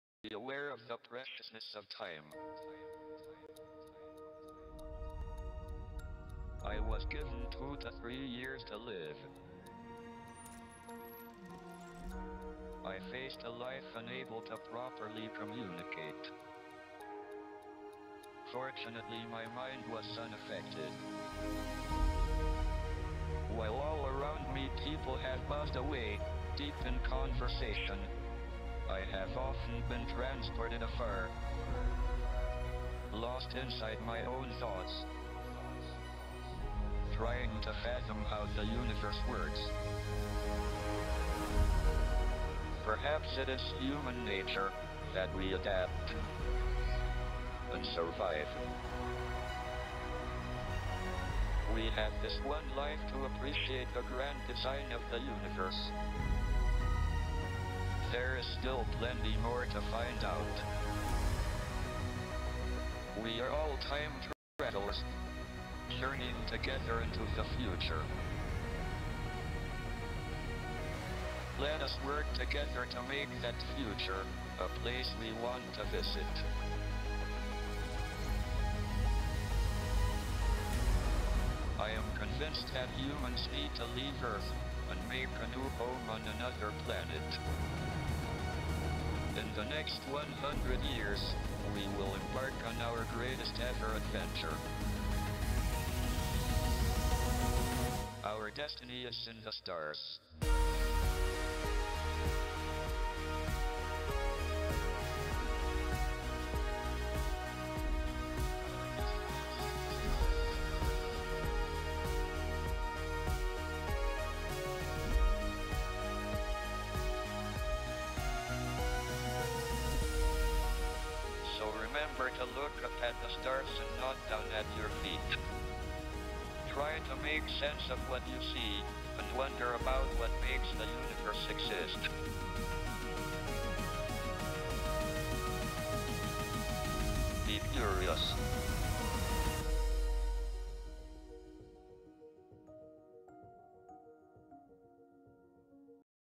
the preciousness of time: Stephen Hawking
Stephen Hawking says about time For future and says about how the universe works?